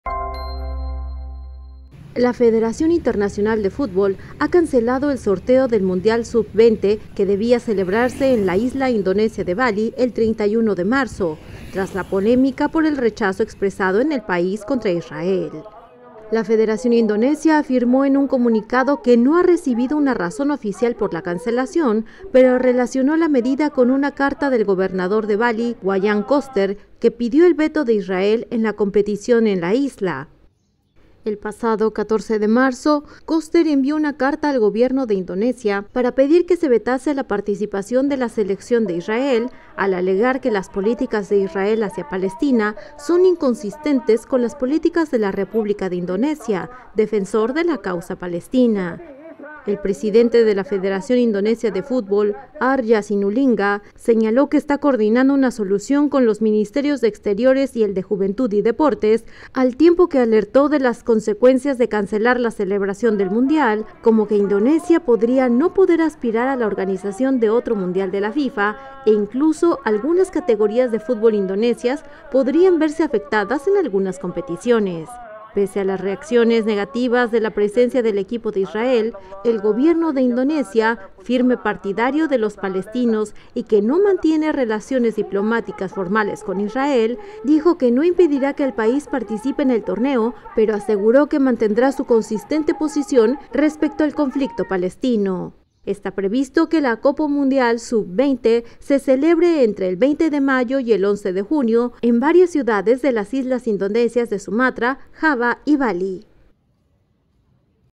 FIFA cancela sorteo del mundial sub 20 en Indonesia tras polémica con Israel
La Federación Internacional de Fútbol (FIFA) ha cancelado el sorteo del mundial sub-20 que debía celebrarse en la isla indonesia de Bali el 31 de marzo tras la polémica.